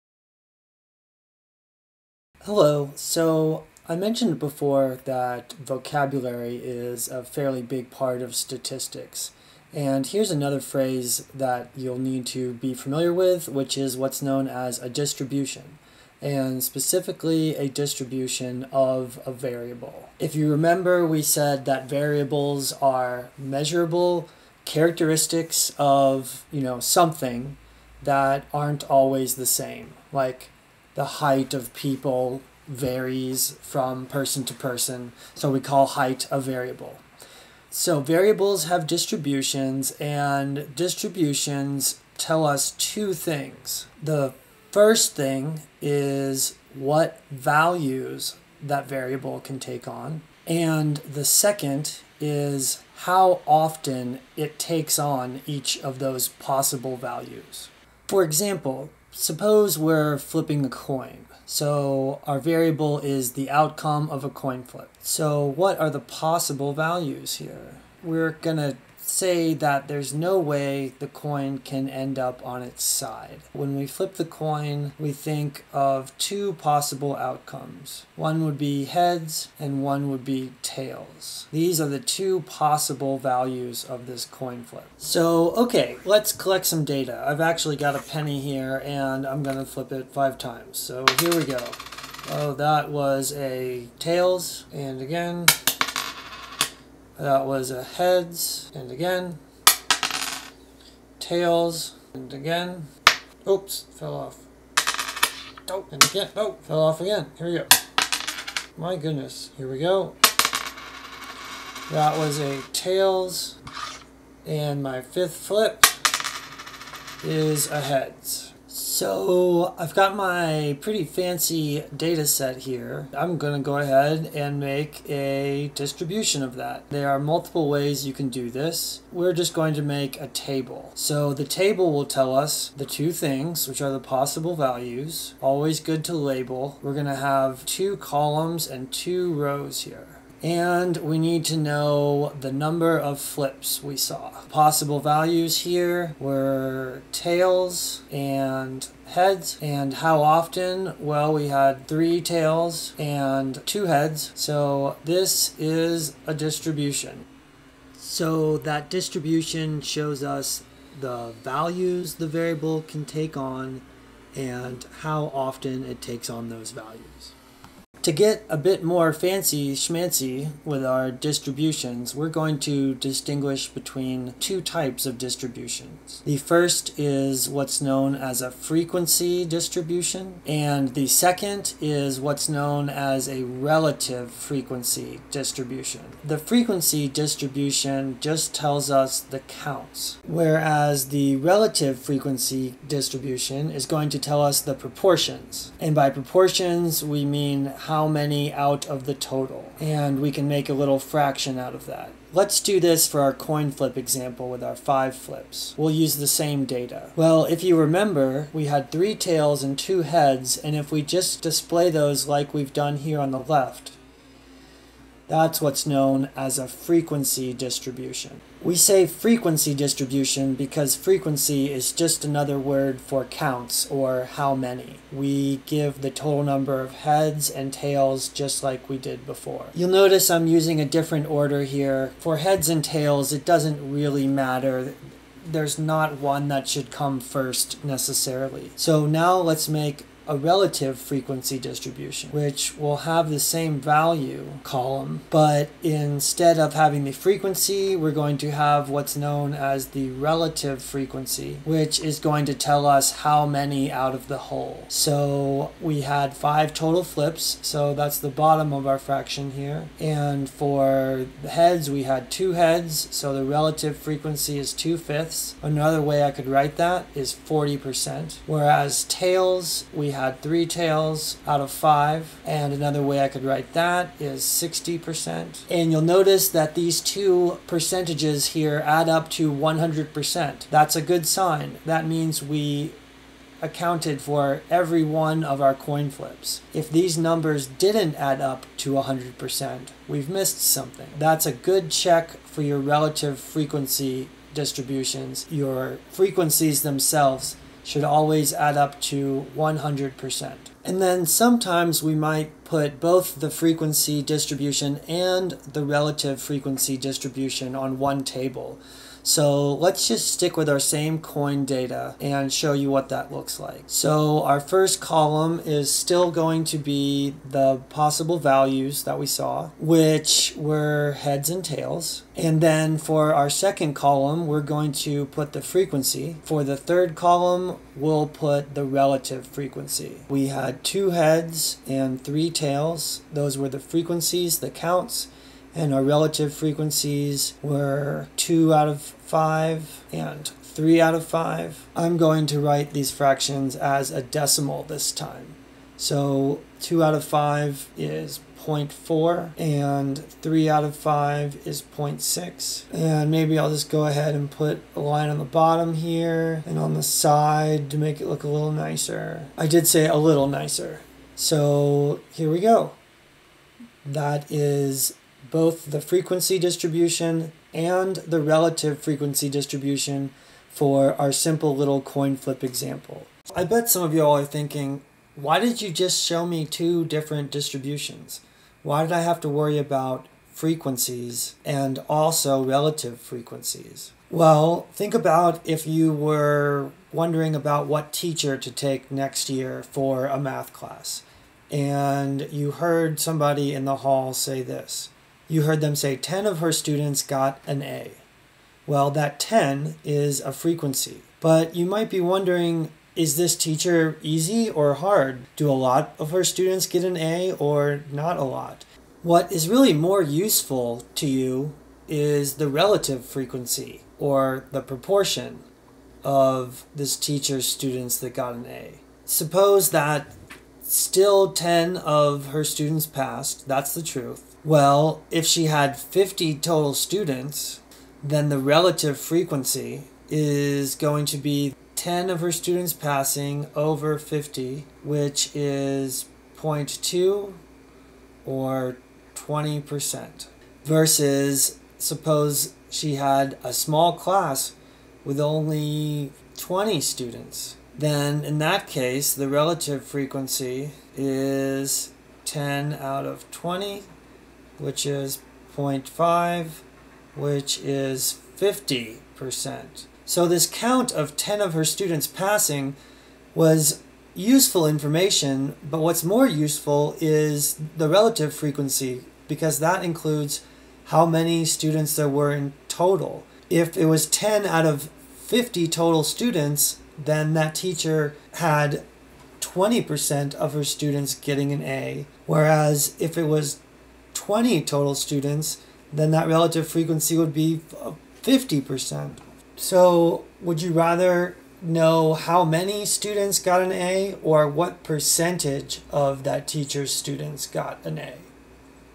Statistics: Distributions of a Discrete Variable: (Frequency and Relative Frequency)
Answers "What is a distribution?" and "What is the difference between frequency and relative frequency distributions?".

00:44 What does a distribution show?
01:28 Super-awesome coin flip data collection
02:11 Making a frequency table
04:07 Making a relative frequency table
06:15 What's the point of relative frequencies? (proportions)